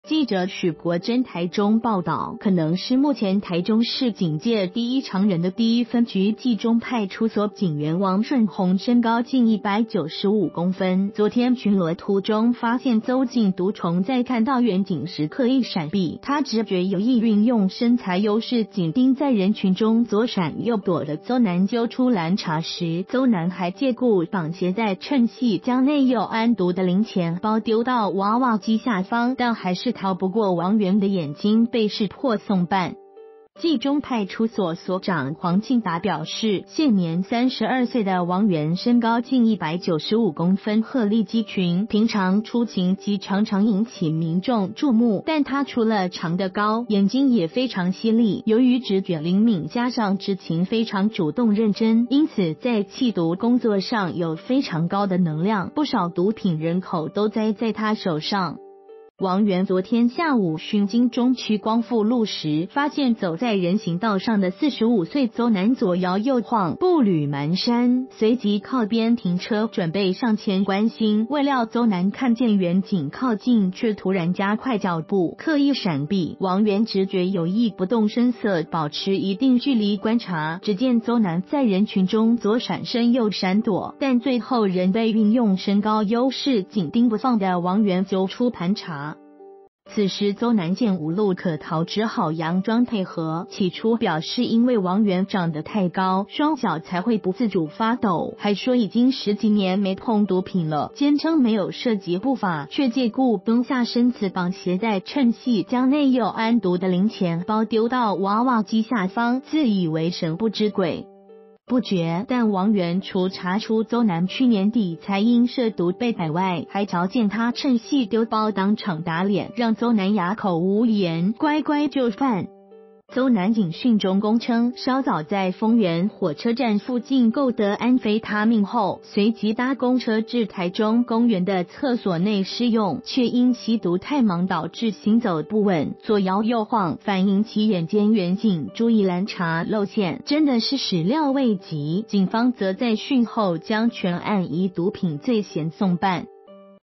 毒蟲人群中左閃右躲 仍被身高195CM警察揪出
〔記者許國楨／台中報導〕可能是目前台中市警界第一長人的第一分局繼中派出所警員王順弘身高近195公分，昨天巡邏途中發現鄒姓毒蟲在看到員警時刻意閃避，他直覺有異運用身材優勢緊盯在人群中左閃右躲的鄒男揪出攔查時，鄒男還藉故綁鞋帶趁隙將內有安毒的零錢包丟到娃娃機下方，但還是逃不過王員的眼睛被識破送辦。

發表評論評論如下。